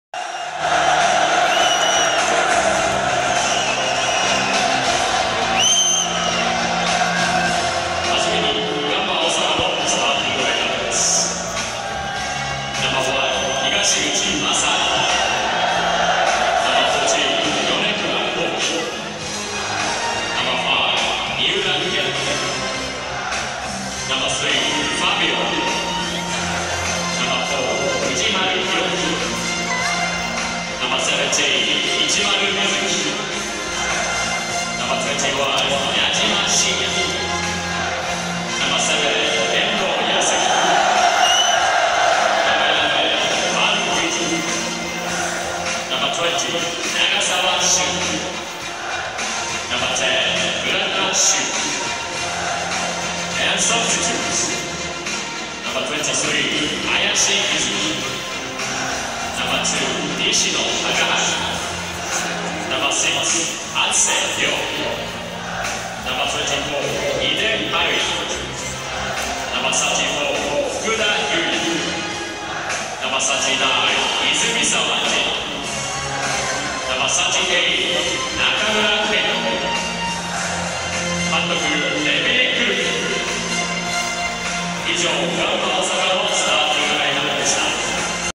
ガンバ大阪スタメン発表 鹿島アントラーズVSガンバ大阪
ガンバ大阪スタメン発表
2018 明治安田生命 J1リーグ 第2節
鹿島アントラーズ 1-0 ガンバ大阪
Kashima Antlers 1-0 Gamba Osaka